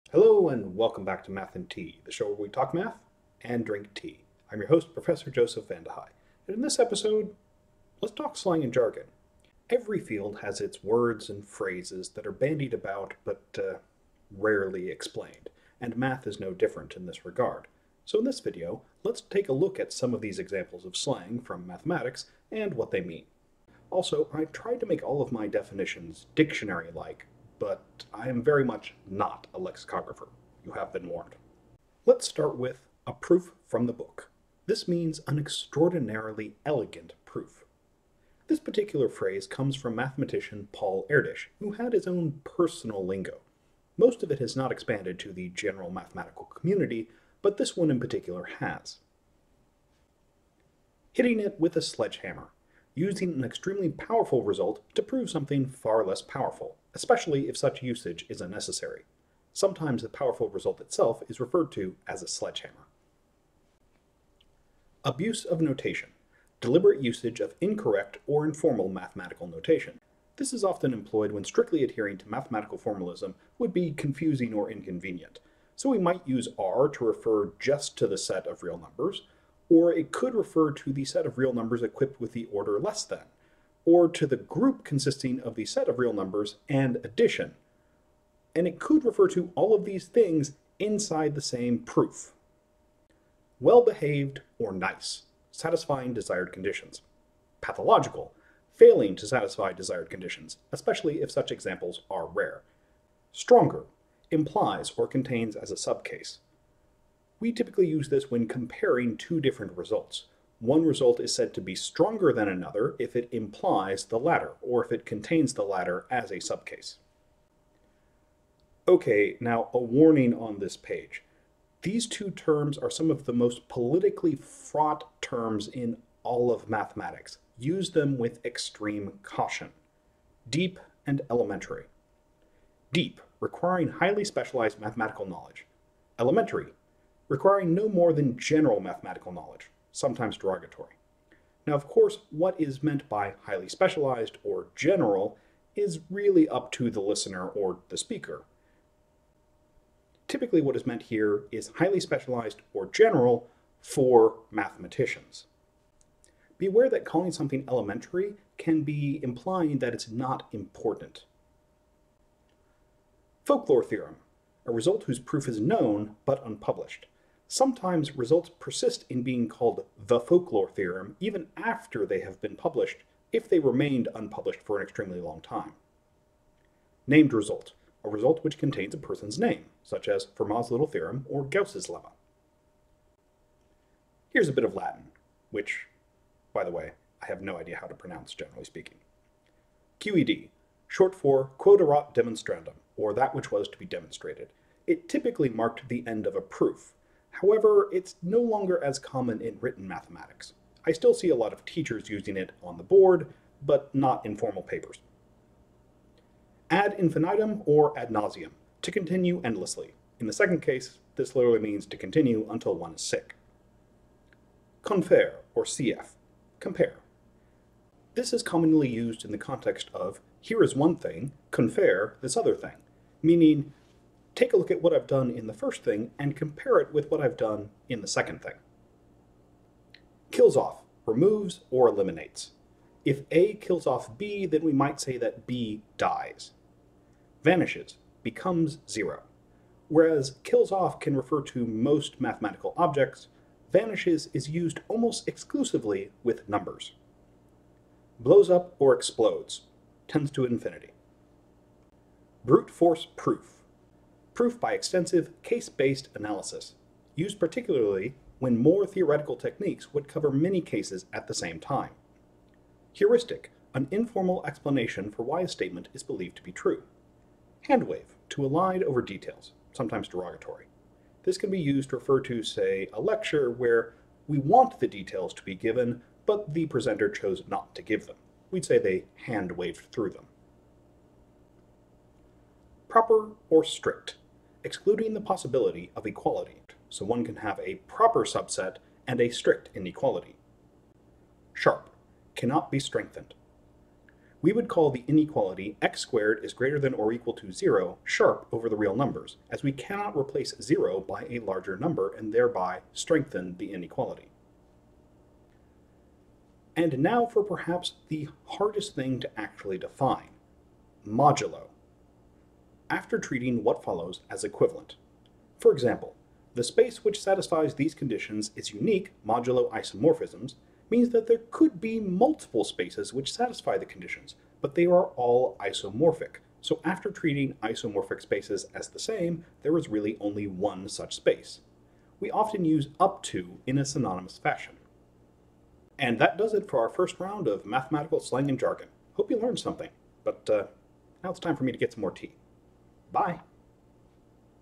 Math slang and jargon
Ever wondered what mathematicians mean when they talk about sledgehammers, modulo, or vanishing? In this video, we give a quick run down of commonly used slang and jargon in the mathematical community.